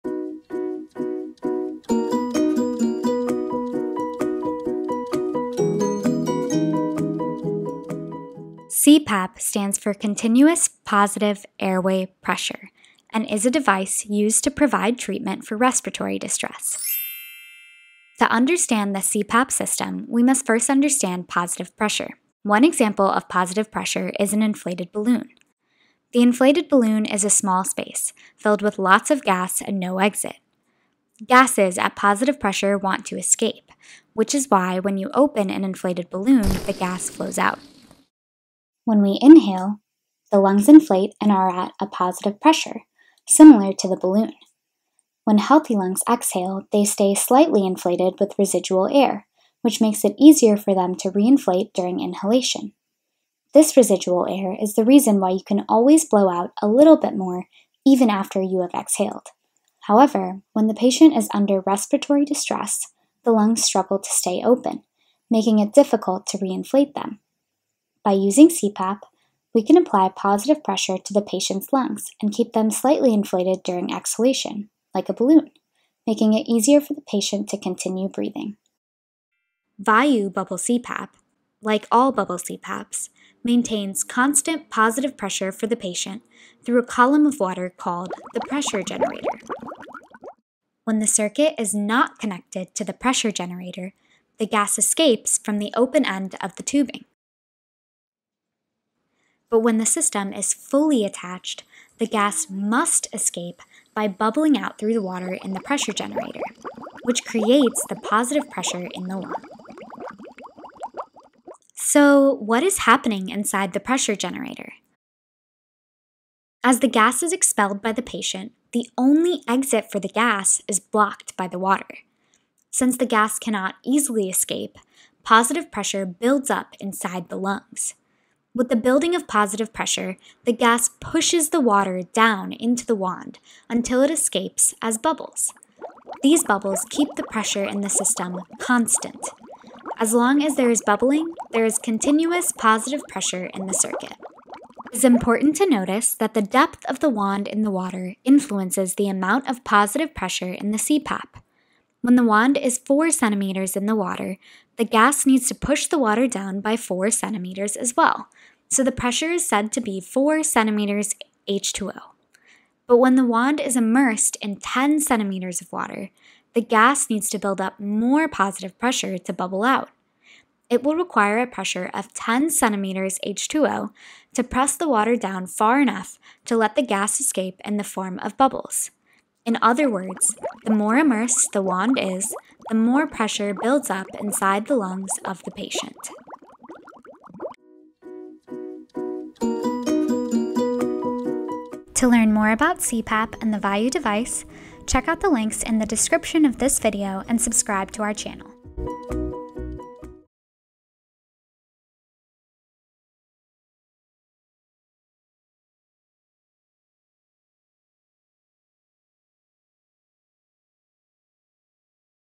Vayu Bubble CPAP: how pressure works
This video is about how pressure is generated in the Vayu bCPAP system.